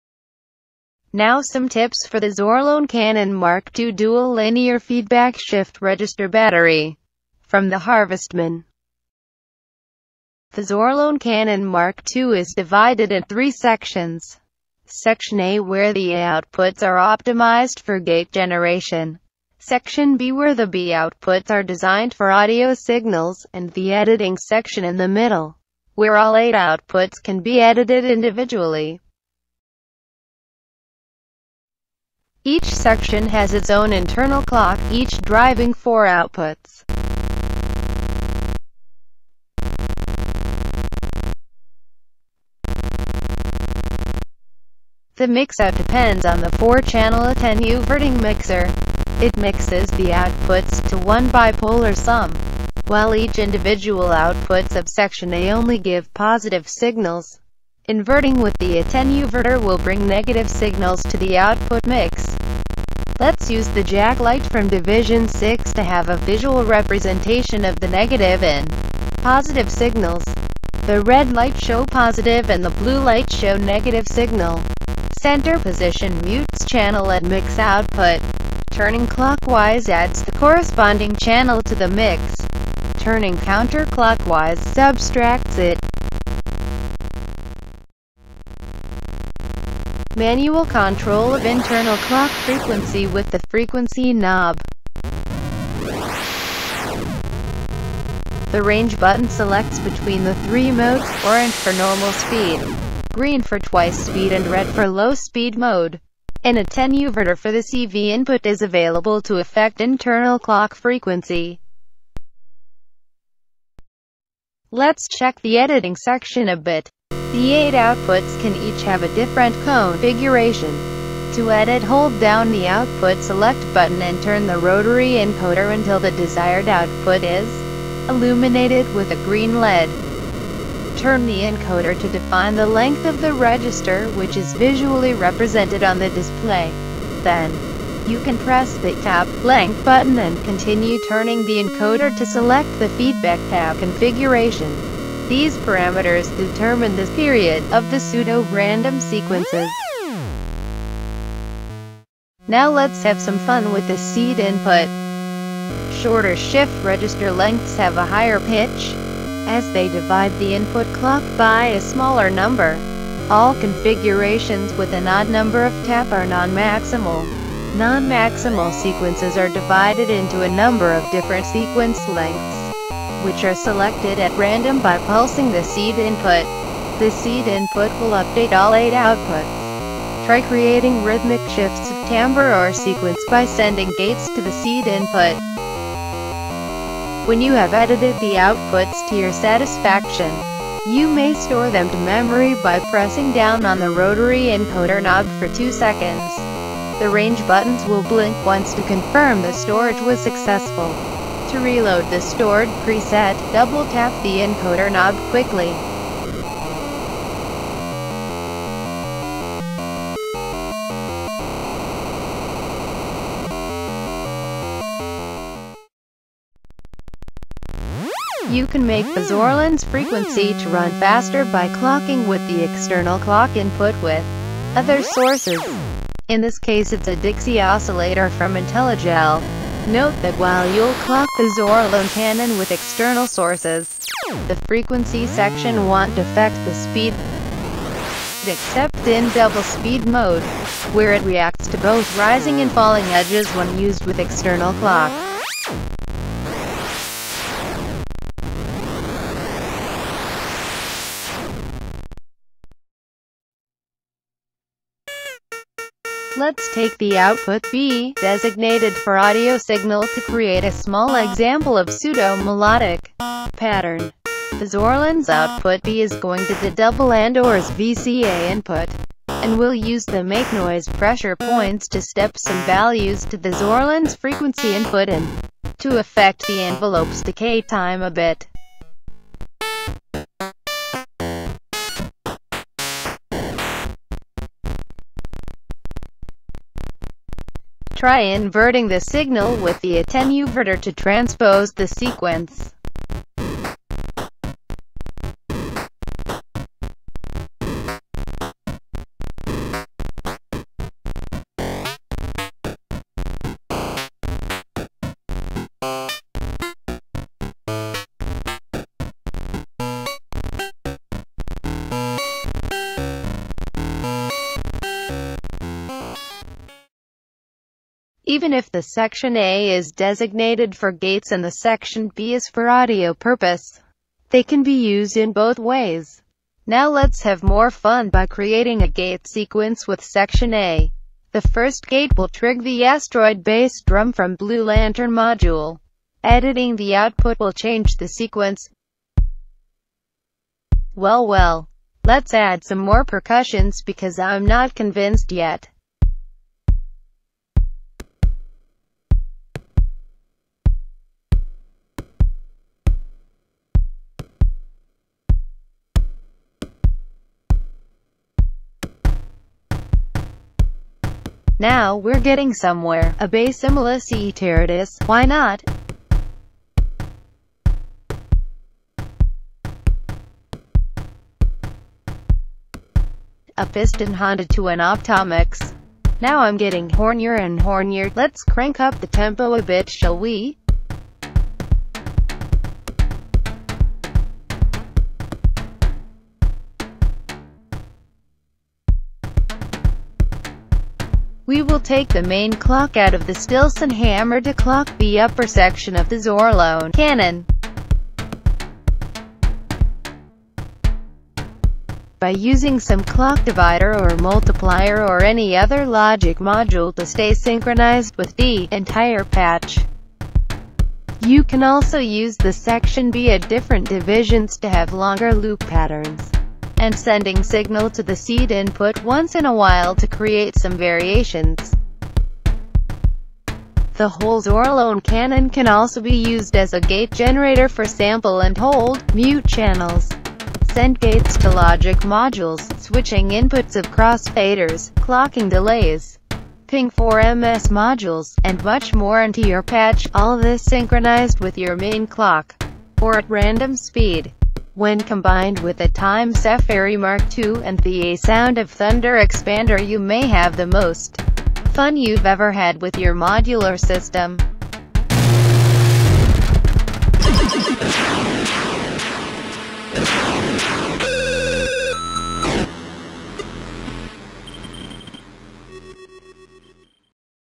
Zorlon Cannon MKII quick demo & Tips from The Harvestman
Quick explenazitions of the Zorlon Cannon MKII.